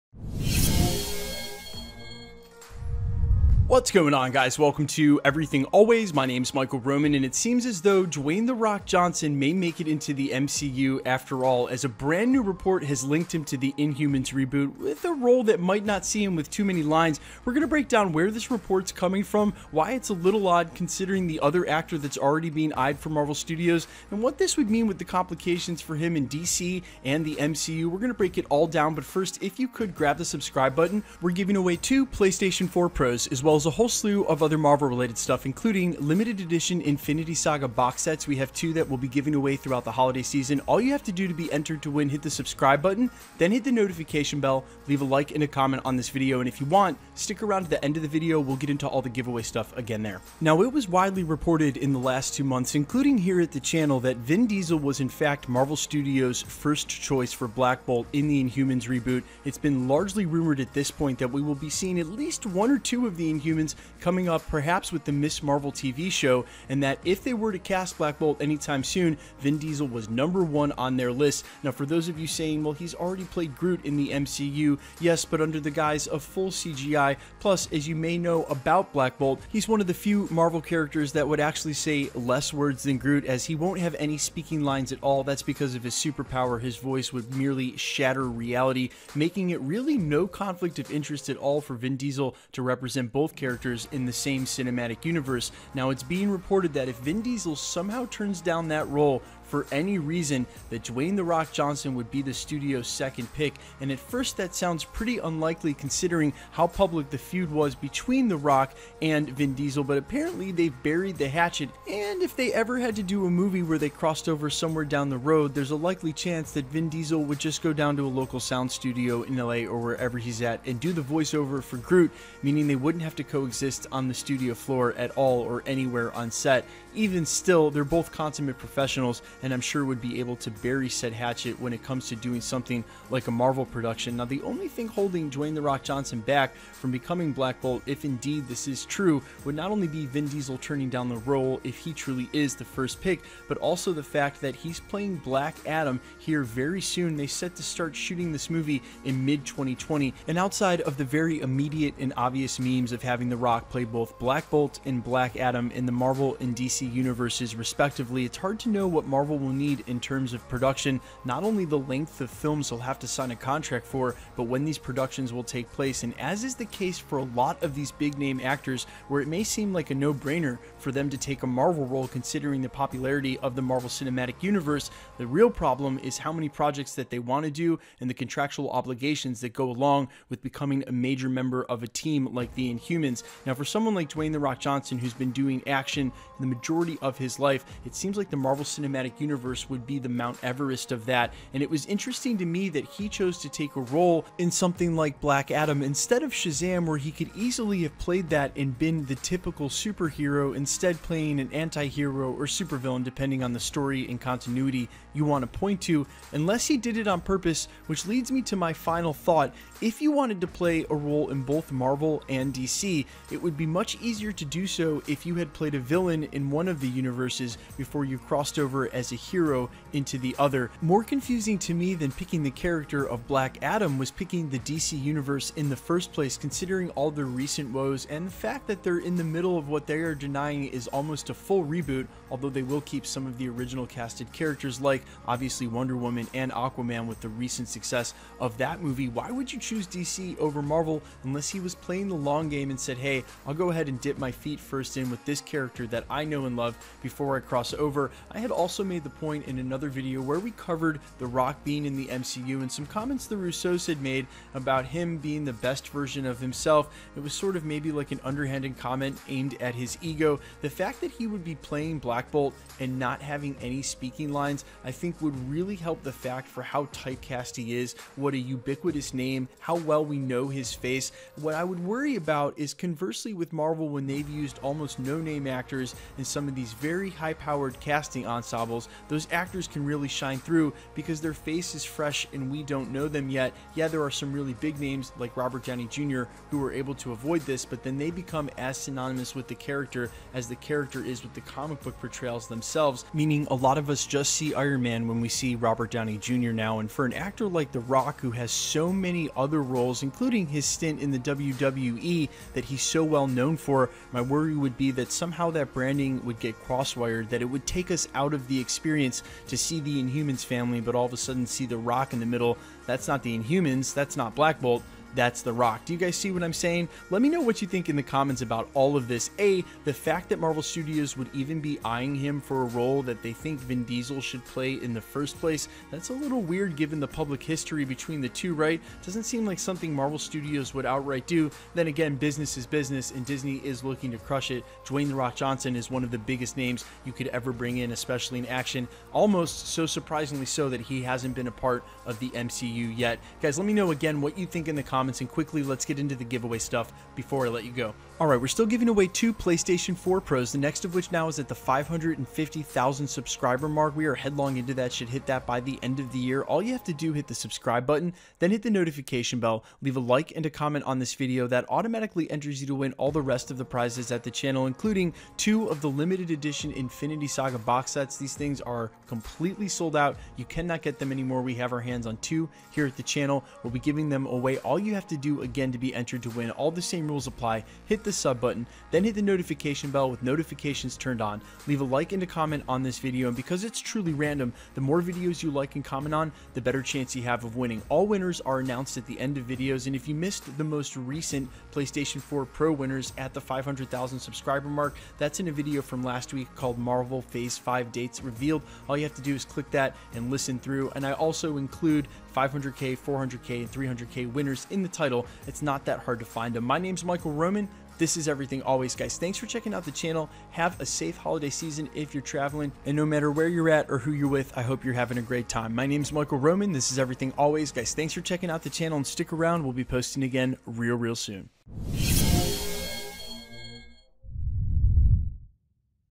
DWAYNE THE ROCK JOHNSON AS BLACK BOLT IN THE MCU?
a new report links Dwayne The Rock Johnson to the MCU, and suprisingly to a role that has been eyeing Vin Diesel. Black Bolt in the heavily rumored Inhumans reboot that all but looks like a shoe in at this point.. today we break that story down...